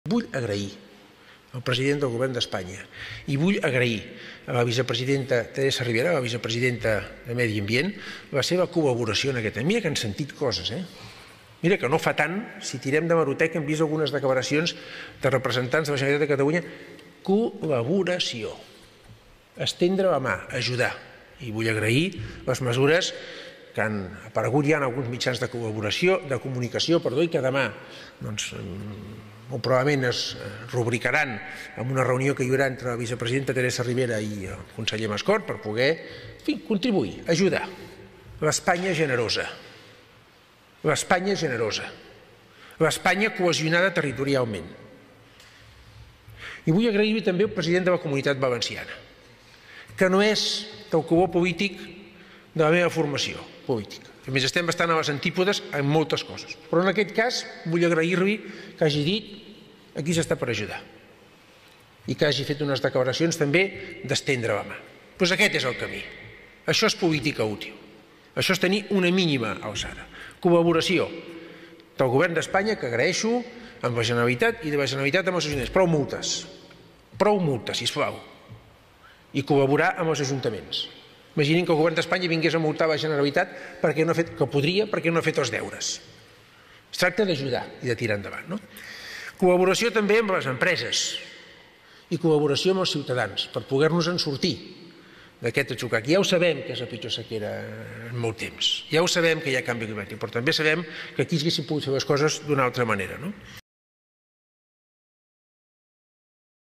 Illa agradece la ayuda de Mazón con la sequía
Illa (PSC) agradece que Mazón esté "para ayudar" ante el traslado de agua de Sagunto a Cataluña.